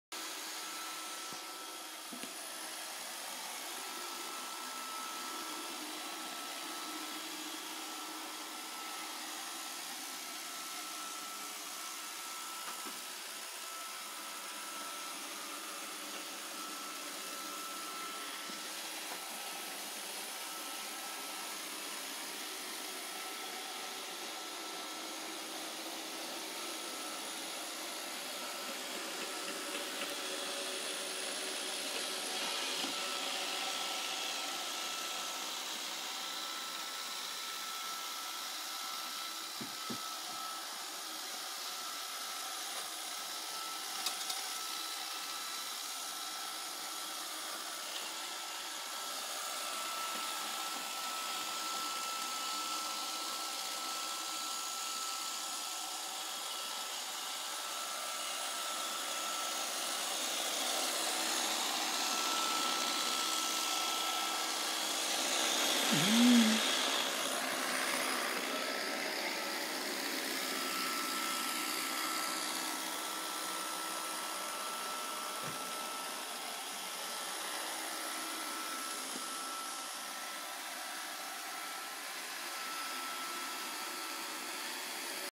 Mein Saugroboter iRobot 530 in Aktion
Gestern angekommen musste der Saugroboter gleich an die Arbeit. Macht Spass beim zuschauen.